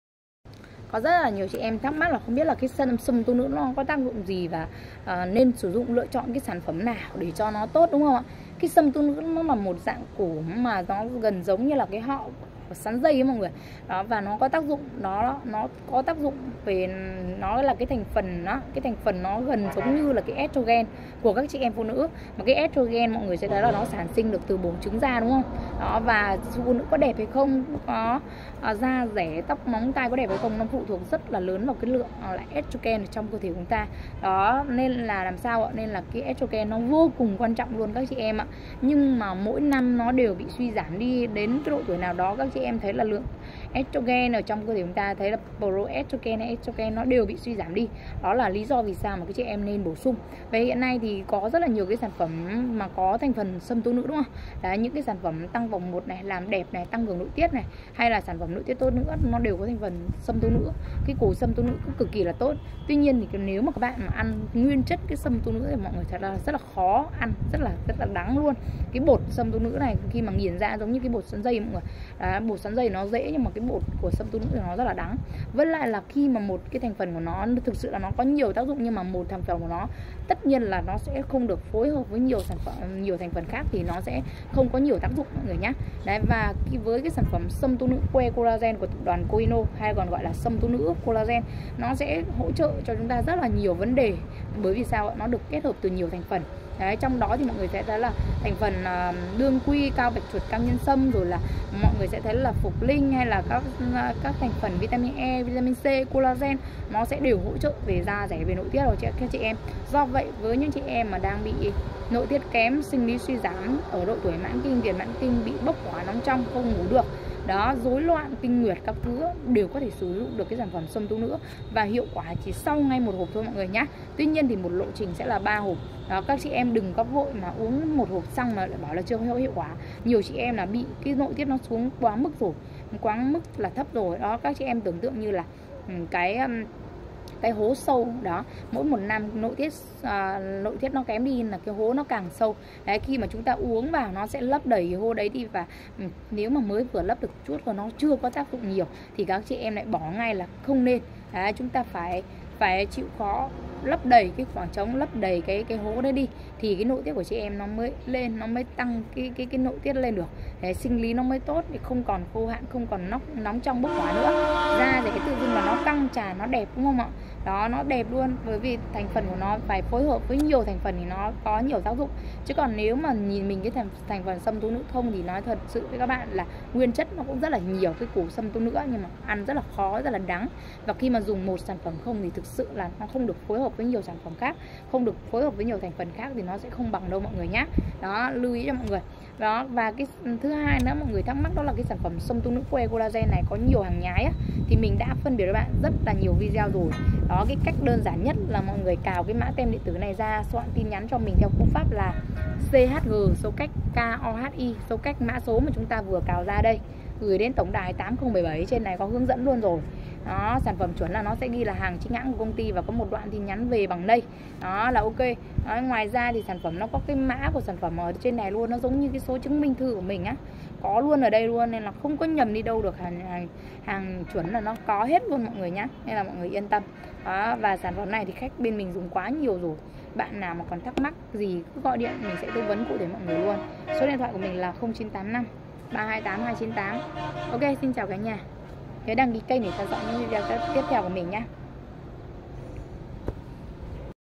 Tác dụng của Sâm Tố Nữ Collagen | Đối tượng nên sử dụng Sâm Tố Nữ Collagen
* Chào bạn , mình là Nguyễn Loan số điện thoại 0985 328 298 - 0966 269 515
* Trong video này mình sẽ một lần nữa giới thiệu bạn sản phẩm Sâm Tố Nữ Kohinoor nhé.